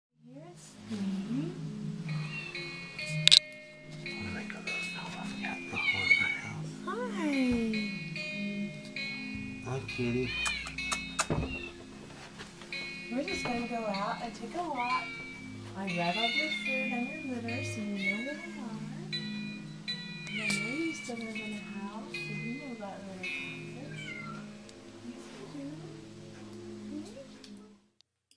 Chaplin meeting Mickey for the very first time - February 5th, 2011
Chaplin crouching under the Rhodes.
Mickey telling him she knows he was in a house.
I am calling him Kitty, and not sure of his origin, sex or age.
Freshest document of Chap, except for a couple images.